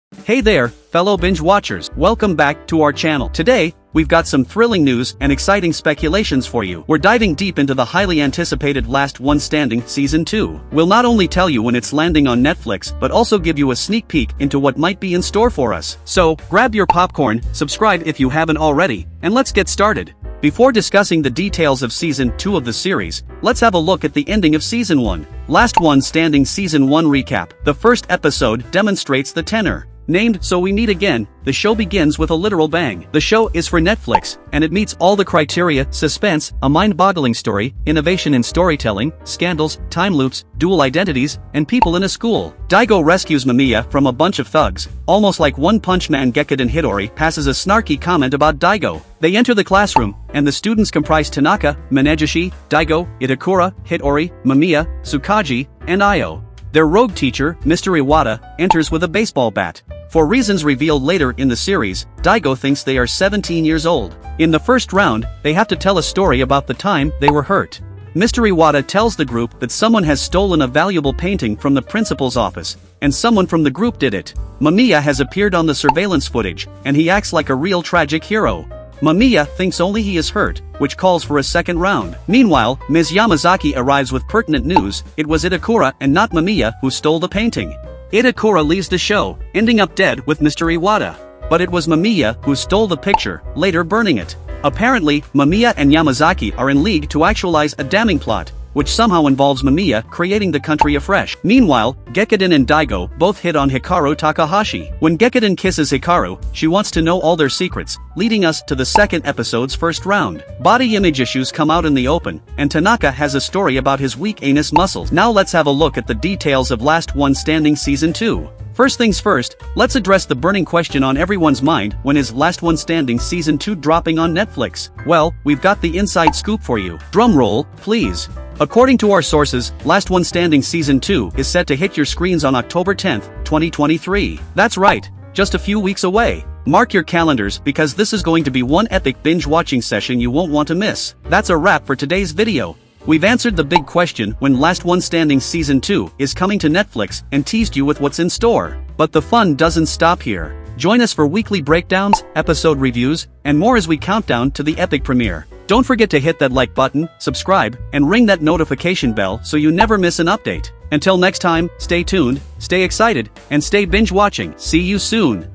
Last One Standing Season 2 Release Date, Cast and More
Hey there, fellow binge-watchers! Today, we've got some thrilling news and exciting speculations for you. We're diving deep into the highly anticipated "Last One Standing" Season 2. We'll not only tell you when it's landing on Netflix but also give you a sneak peek into what might be in store for us.